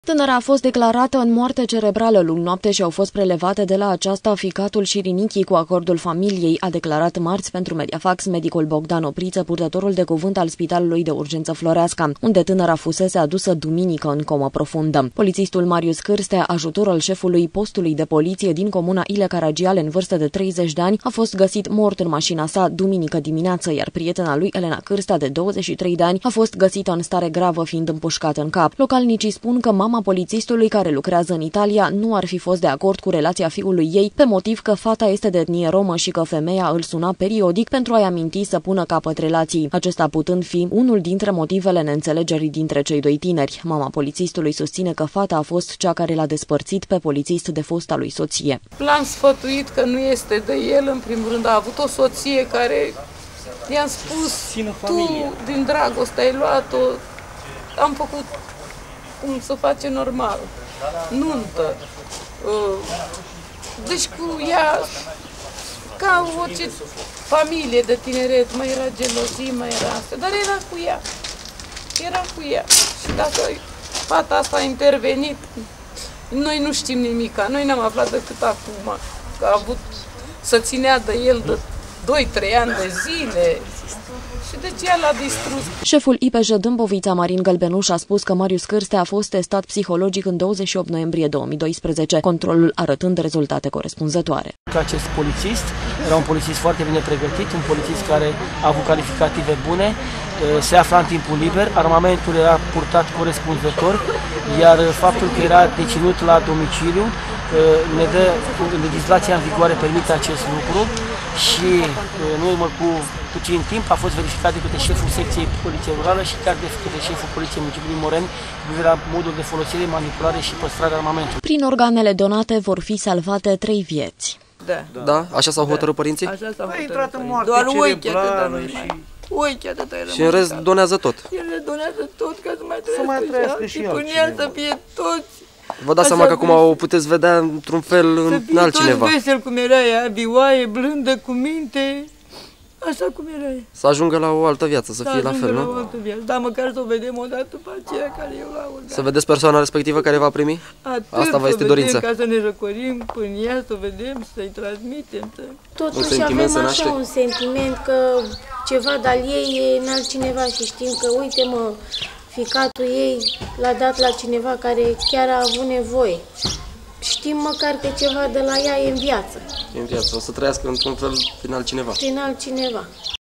TANARA IMPUSCATA DE POLITIST A MURIT (Columna Tv)
Tanara impuscata de politistul din localitatea I.L Caragiale care apoi s a sinucis, a decedat aseara. Familia acesteia si a dat acordul pentru prelevarea ficatului si rinichilor, interventiile fiind realizate la Institutul Fundeni. Tânara a fost declarata în moarte cerebrala luni noapte si au fost prelevate de la aceasta ficatul si rinichii, cu acordul familiei, a declarat marti, pentru MEDIAFAX, medicul Bogdan Oprita, purtatorul de cuvânt al Spitalului de Urgenta Floreasca, unde tânara fusese adusa duminica, în coma profunda.
Politistul Marius Cârstea, ajutor al sefului postului de Politie din comuna I.L. Caragiale, în vârsta de 30 de ani, a fost gasit mort în masina sa, duminica dimineata, iar prietena lui, Elena Pârsa, de 23 de ani, a fost gasita în stare grava, fiind împuscata în cap.
Localnicii spun ca mama politistului, care lucreaza în Italia, nu ar fi fost de acord cu relatia fiului ei pe motiv ca fata este de etnie roma si ca femeia îl suna periodic pentru a-i aminti sa puna capat relatiei, acesta putând fi unul dintre motivele neîntelegerii între cei doi tineri. Mama politistului sustine ca fata a fost cea care l a despartit pe politist de fosta lui sotie.